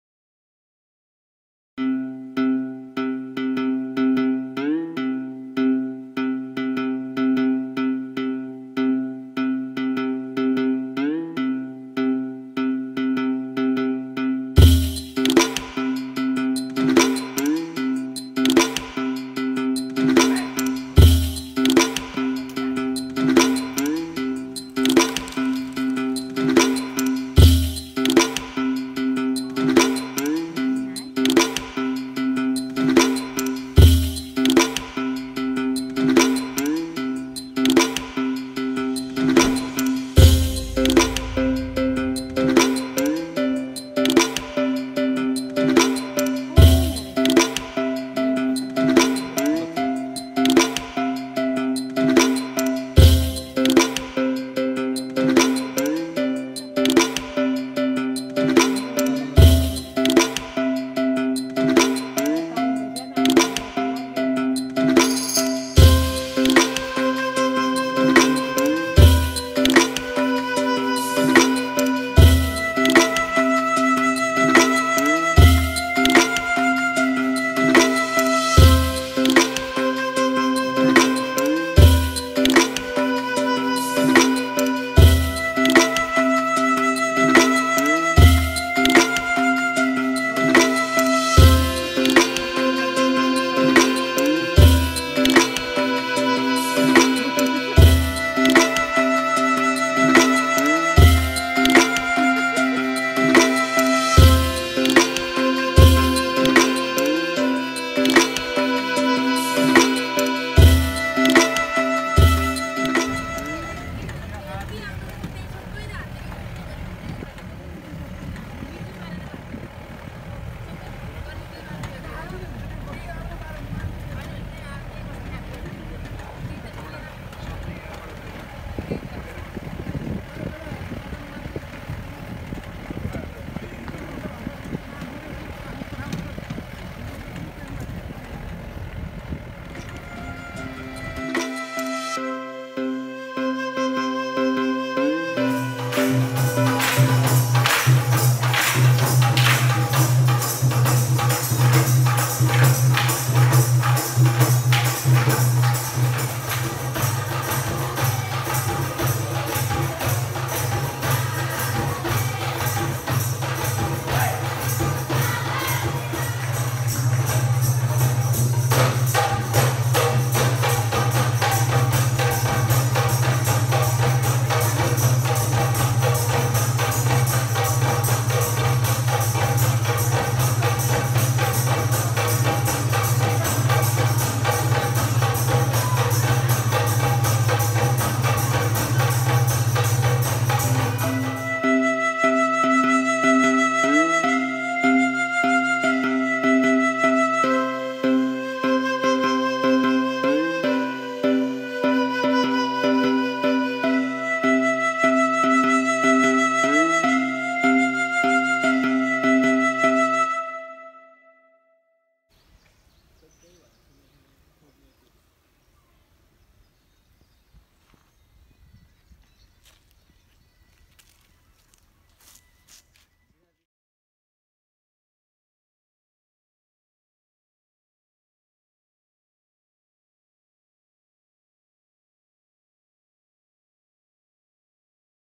Exploring chitwan National park Nepal
Chitwan national park is first  National park of Nepal established in 1973 AD . where we can have a look of various species of plants, birds and animals.  One horn rhino and tiger are the main attraction.
Rhino population  645 in Nepal  (3000 world wide)  605 in Chitwan
Tiger population 198 in Nepal (3200 globally ) 120 in Chitwan